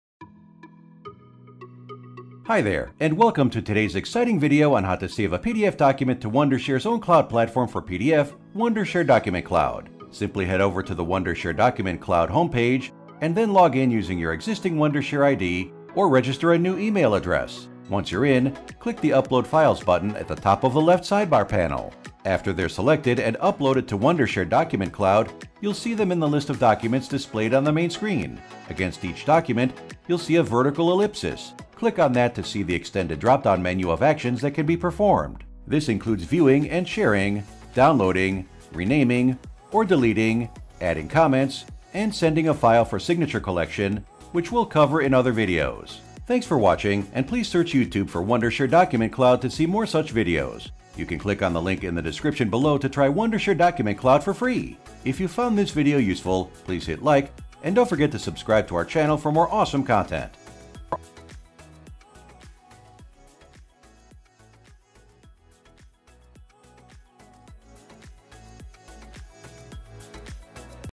How to Save a PDF File to Wondershare Document Cloud
This video is about how to save a PDF file to Wondershare Document Cloud.

🔸Wondershare Document Cloud, embedded in the most complete PDF productivity solution: PDFelement Pro DC - Your PDF workflow in the cloud. Sign, share and collaborate.